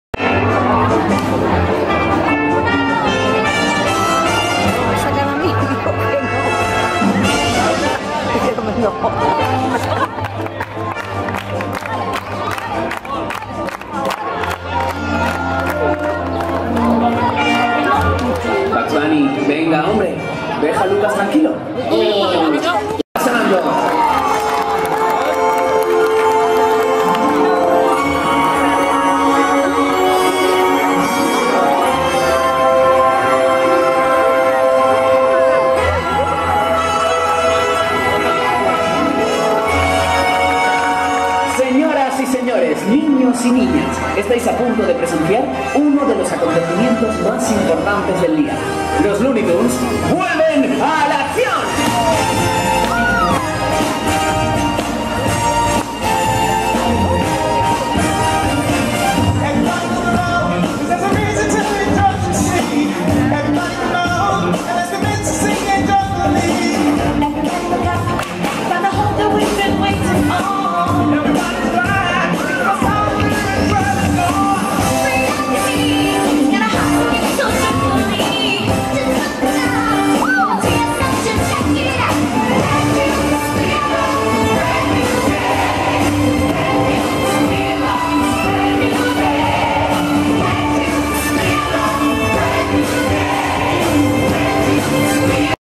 WELCOME SHOW PARQUE WARNER
El cruce de Hollywood Boulevard de Parque Warner Madrid, es el escenario callejero de Welcome Show, un espectáculo de baile y ritmo para dar la bienvenida para los visitantes del parque.  En esta avenida nuestros queridos Looney Tunes celebran la apertura de Parque Warner, entre bailes y canciones que se han preparado para celebrar el comienzo del día a lo grande.